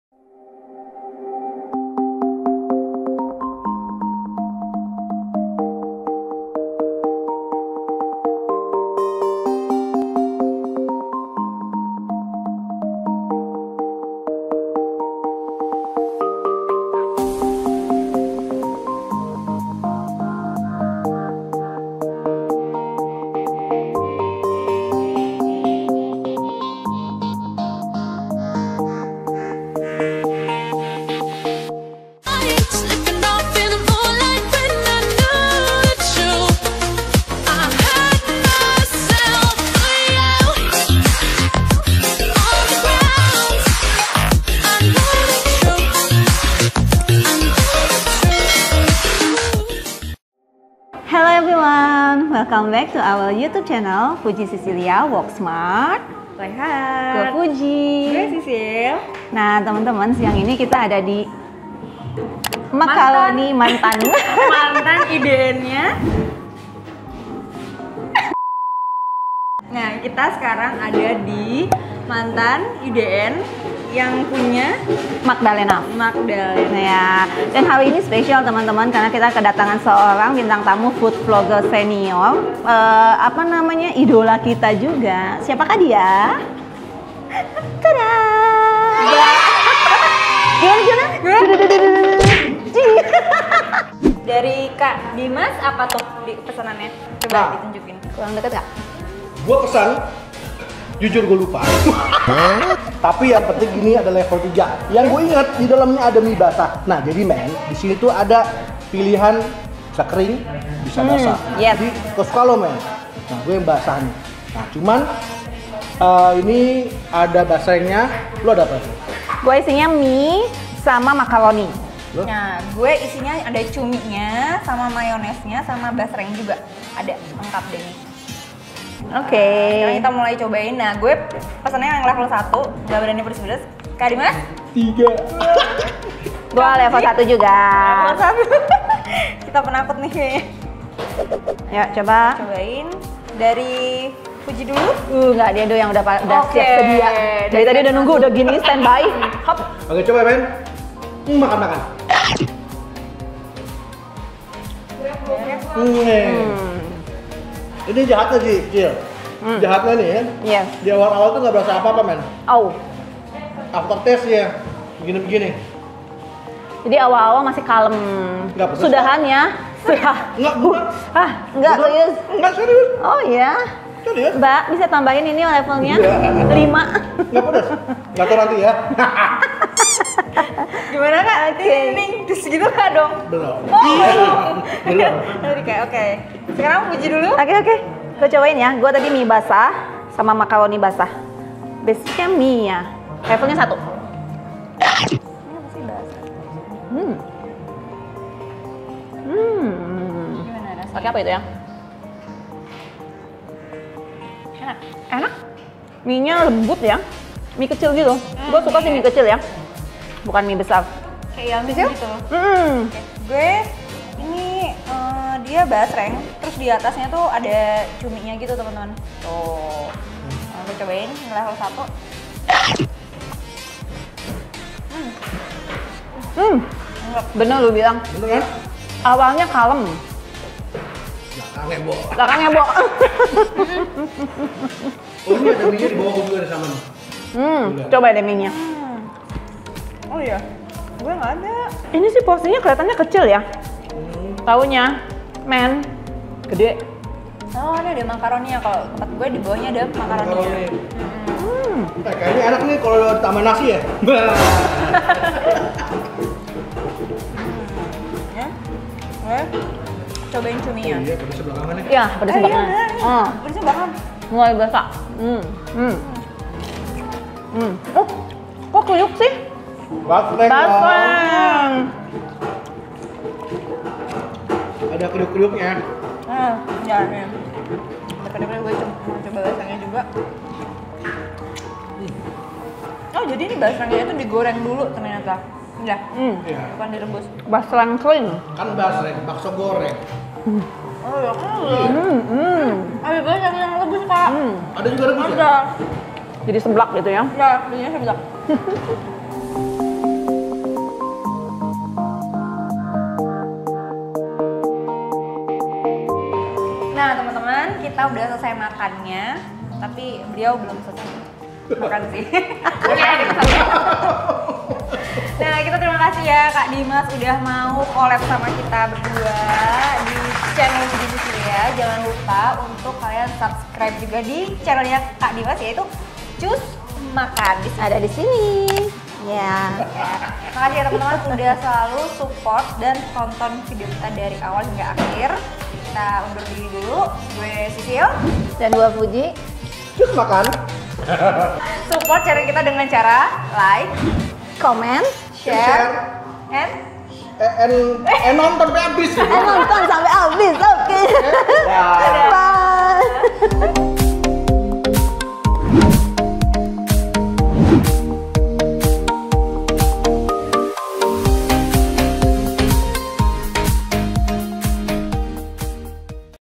BISNIS MAKANAN YOUTUBER | REVIEW MAKANAN YOUTUBER | MANTAN_IDN MAGDALENA
Kuliner food vlogger besar idola kita nih. Mantan, Makaroni Setan. Membekas di lidah dan di hati. Dengan tingkat kepedasan yang berbeda dari 1-3. Menu bisa mix and match, topping nya pun boleh pilih. Ini camilan, tapi kenyang. Penasaran kan ? Yuk nonton video nya.

Hello Everyone !! 👋

Saranghaeyo. Sehat selalu ya buat kalian, dan. selamat menonton video ini!

Happy watching!! 🥰

•Lokasi: Jl. Kemang Raya No.72
•Jam operasional restoran : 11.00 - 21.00
•Range harga mulai Rp. 20.000,-